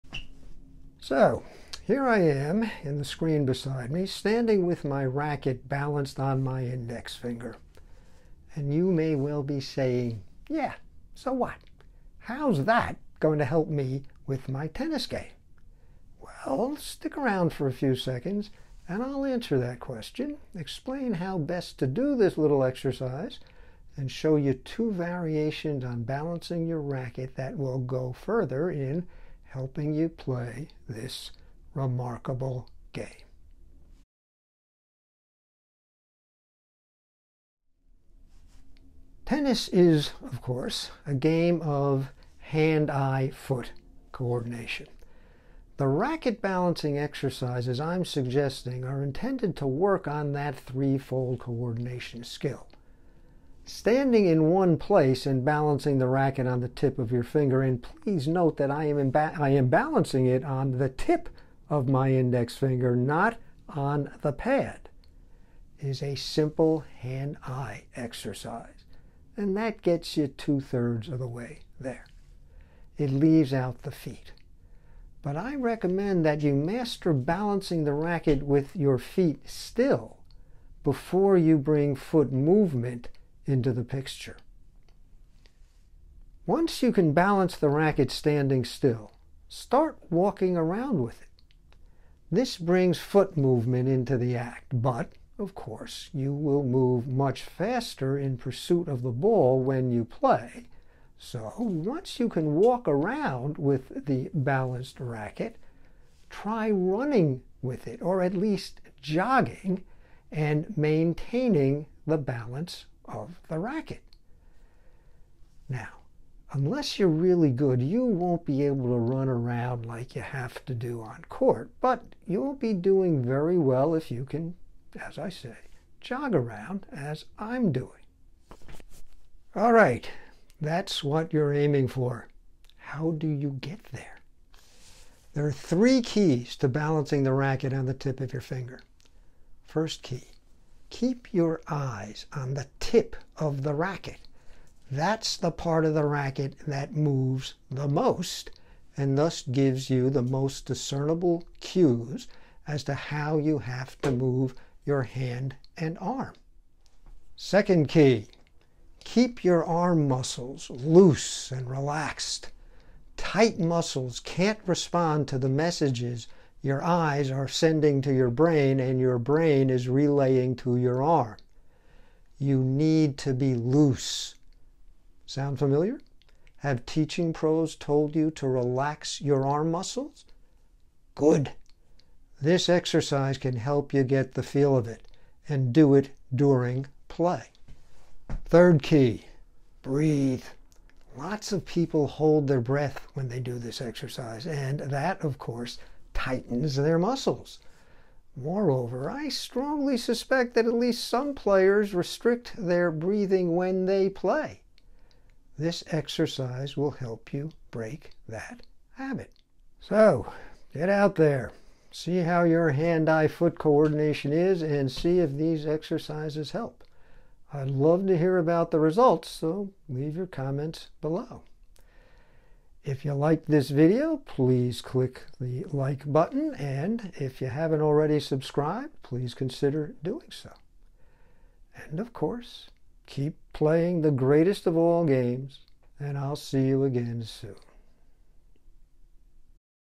Why and How You Should Balance Your Racket
Tennis is a game of hand-eye-foot coordination. This video offers a means of testing and improving that skill.